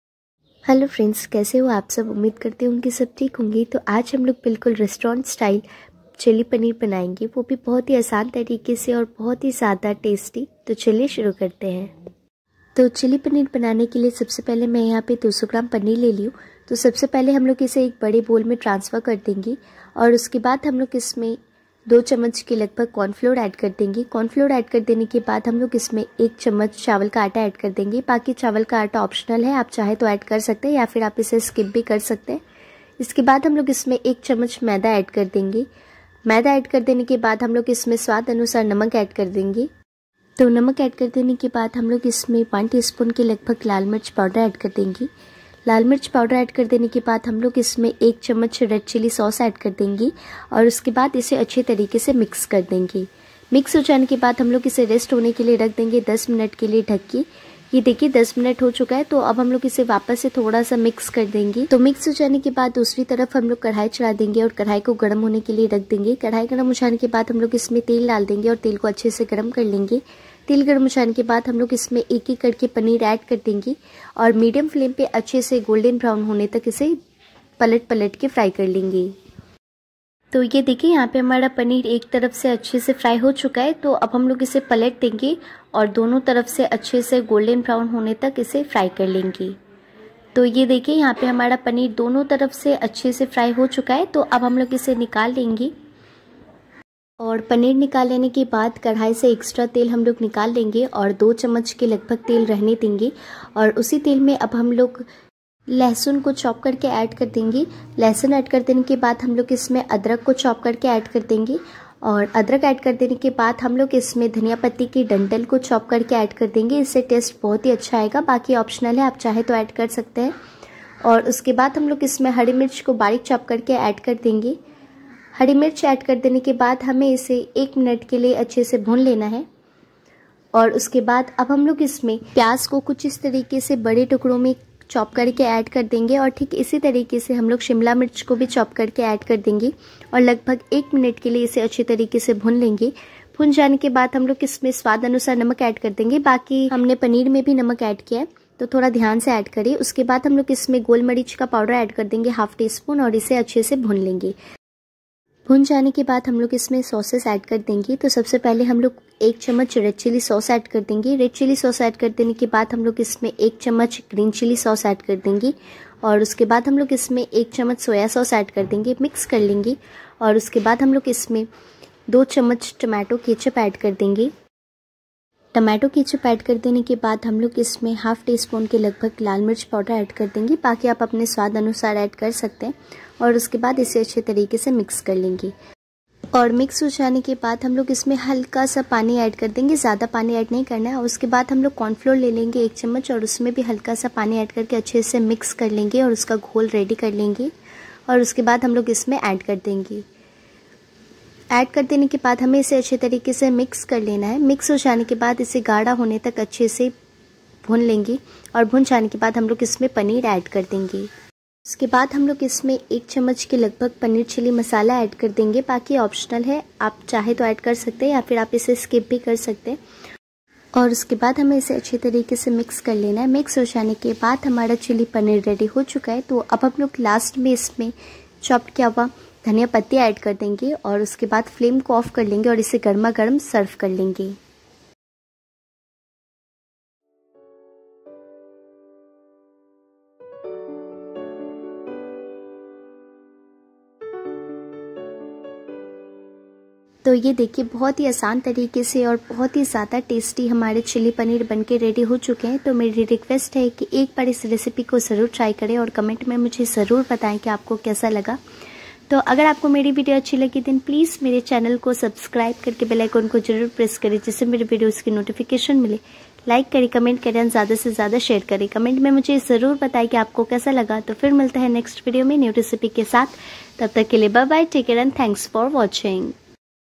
रेस्टोरेंट स्टाइल आसान और टेस्टी चिल्ली पनीर बनाए इस तरीके से |Restaurant Style Chilli Paneer Recipe
Hello, Friends. How are you all? I hope that all are Well and Fine. So Today, We are making very tasty Chilli Paneer.

Ingredients :---

● For Marinating -
• Paneer : 200 gm
• Corn Flour : 2 spoon
• Rice Flour : 1 spoon
• All Purpose Flour : 1 spoon
• Salt : To Taste
• Red Chilli Powder : 1 tsp
• Red Chilli Sauce : 1 spoon

● For Frying -
• Oil : As Required
• Paneer : Marinated

● For Gravy -
• Oil : 2 spoon
• Garlic : Chopped
• Ginger : Choppes
• Coriander Stalk : Chopped
• Green Chilli : Chopped
• Onion : Dicely Chopped
• Capsicum : Dicely Chopped
• Salt : To Taste
• Black Pepper Powder : ½ tsp
• Green Chilli Sauce : 1 spoon
• Soya Sauce : 1 spoon
• Tomato Ketchup : 2 spoon
• Red Chilli Powder : ½ tsp
• Corn Flour : 1spoon
• Paneer Chilli Masala : 1 spoon
• Coriander Leaves : Chopped